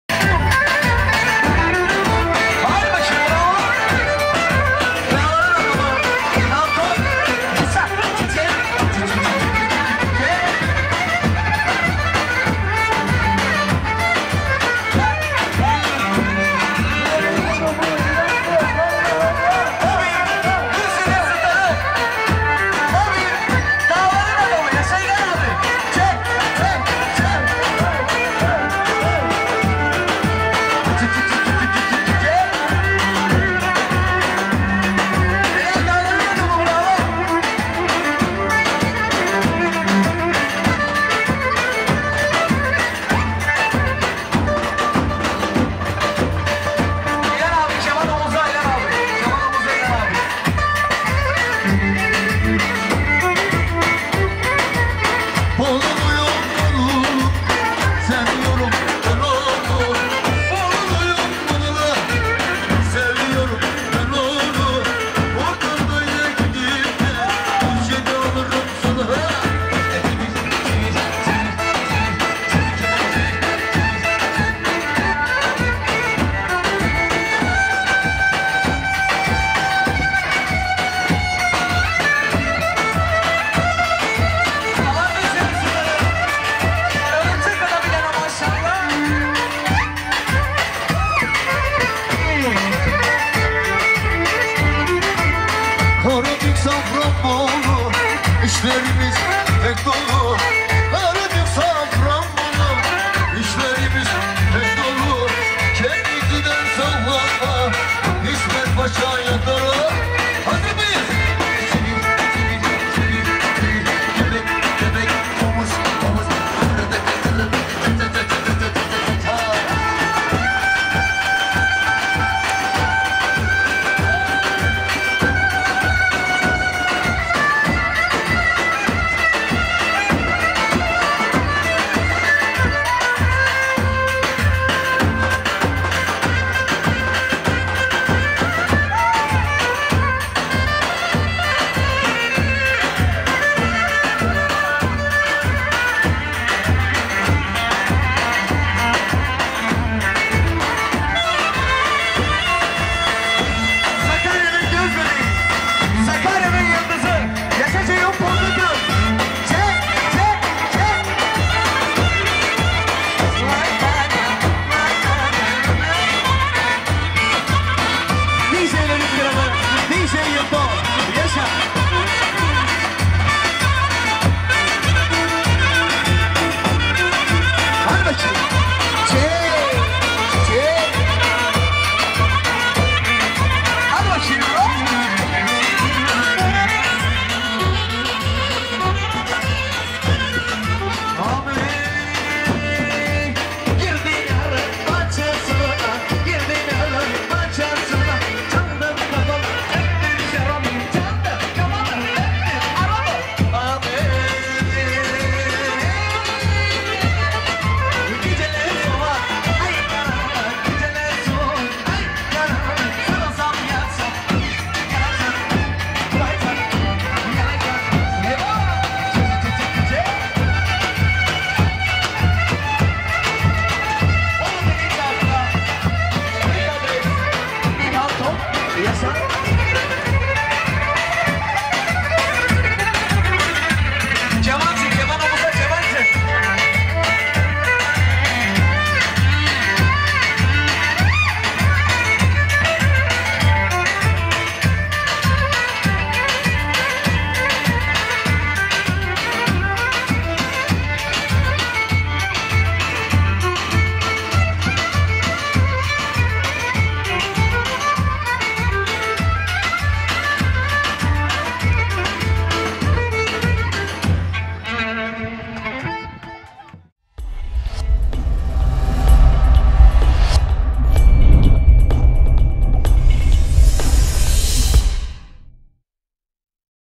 Roman Havası TV ☆ ♫ █▬█ █ ▀█▀ ♫ ÇILGIN KEMANACI İLHAN DAN BOLU SALLAMA
müthiş Bolu Sallaması Okadar Müthişki Kemancı Bile Yerinde
   Duramıyor O derce yani Grup Sakarya nın Çılgın Kemancısı
kurt Prodüksiyon Kurt Prodüksiyon SAKARYA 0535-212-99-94